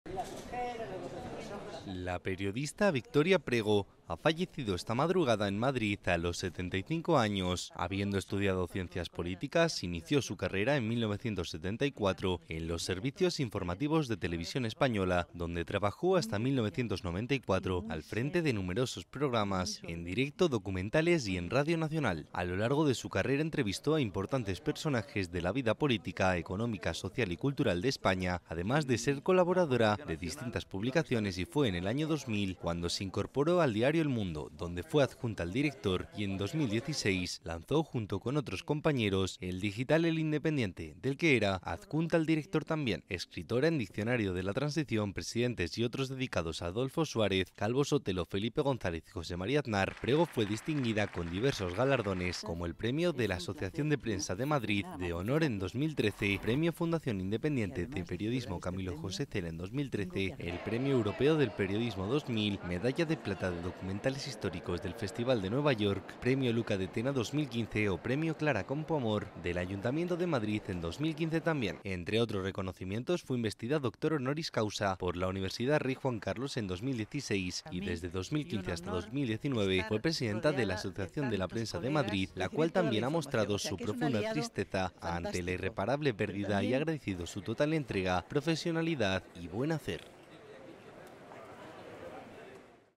Muere la periodista Victoria Prego, voz de la Transición española, a los 75 años
La periodista Victoria Prego ha fallecido esta madrugada en Madrid a los 75 años.   Prego estudió Ciencias Políticas e inició su carrera en 1974 en los servicios Informativos de TVE, donde trabajó hasta 1994 al frente de numerosos programas en directo, documentales y en RNE.

(Fuente: Europa Press / APM / URJC)